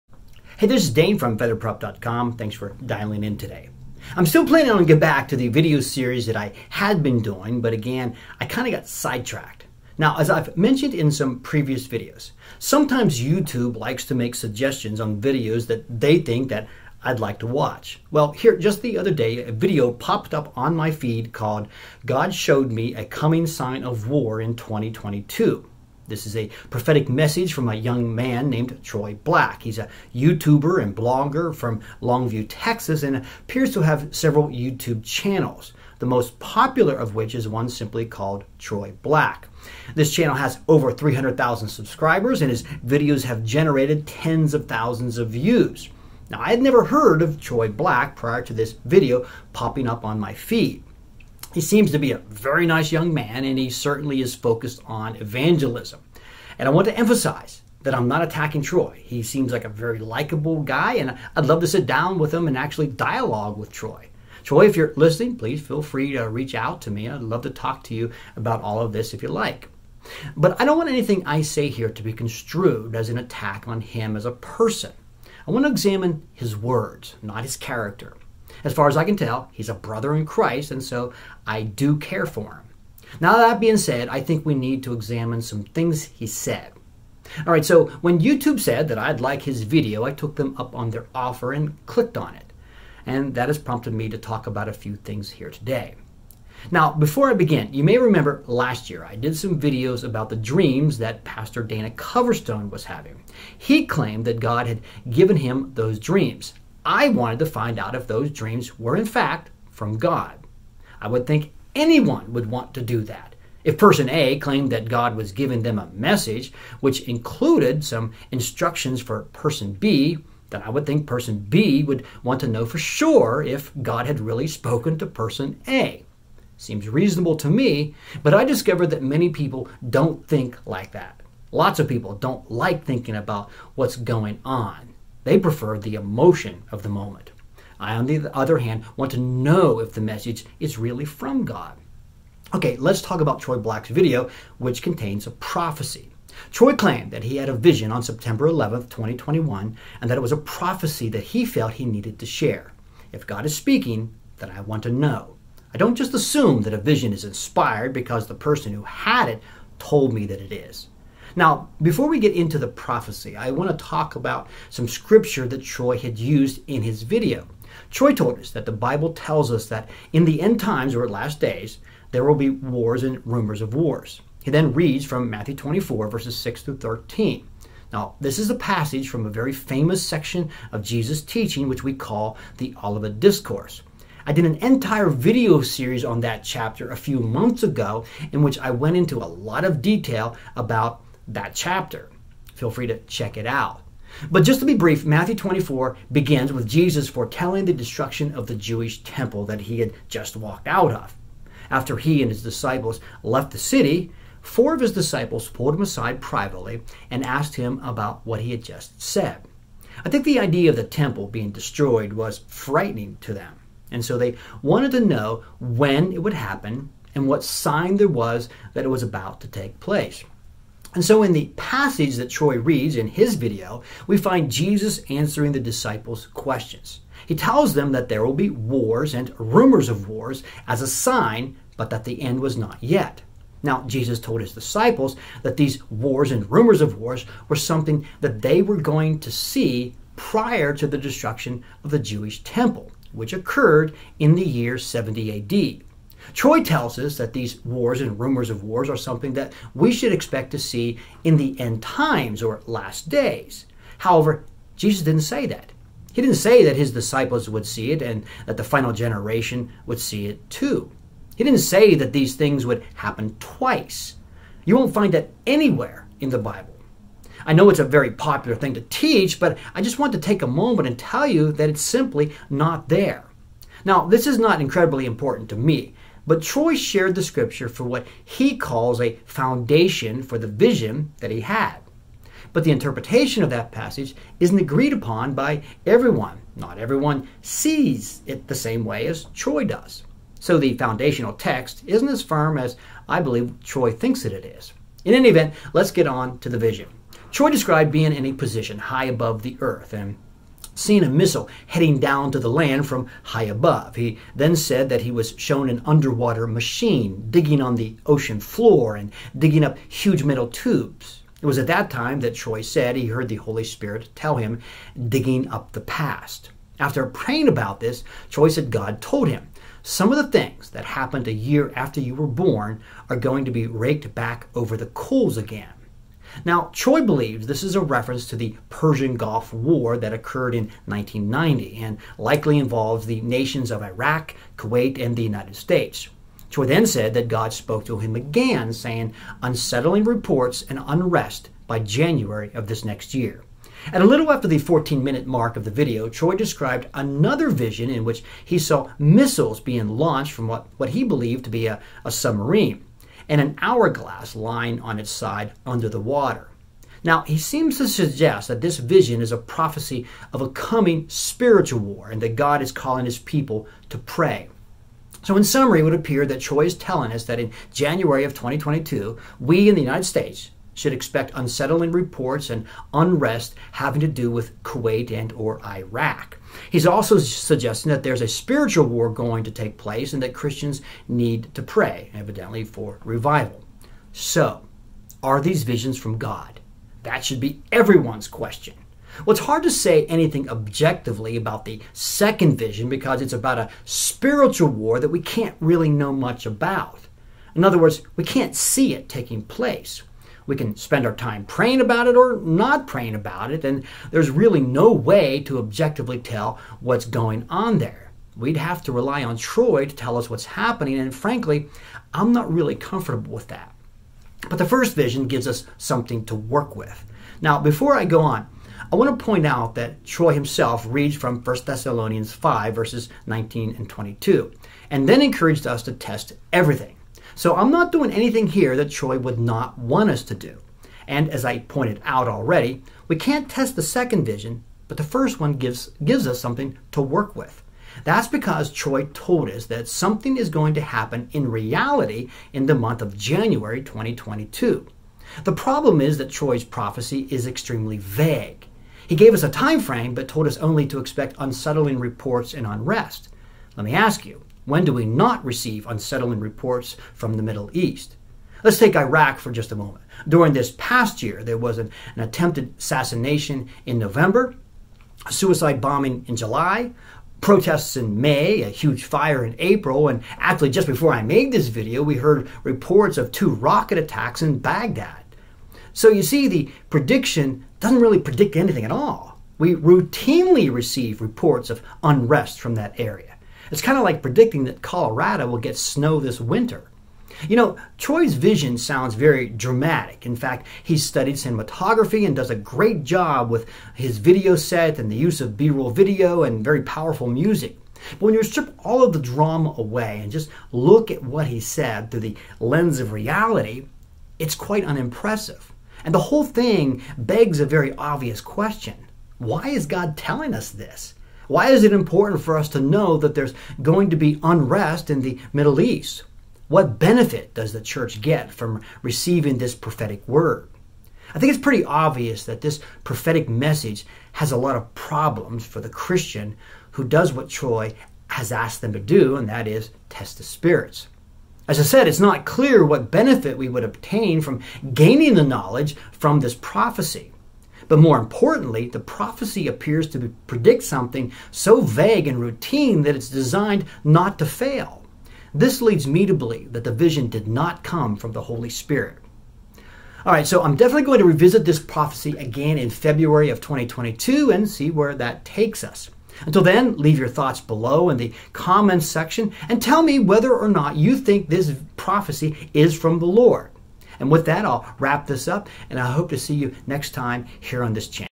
Examining Troy Black's "War" Prophecy for 2022
In December 2021, Troy Black posted a video concerning a prophetic message for 2022 which regards a "war." In this video I want to examine some of the things that he said.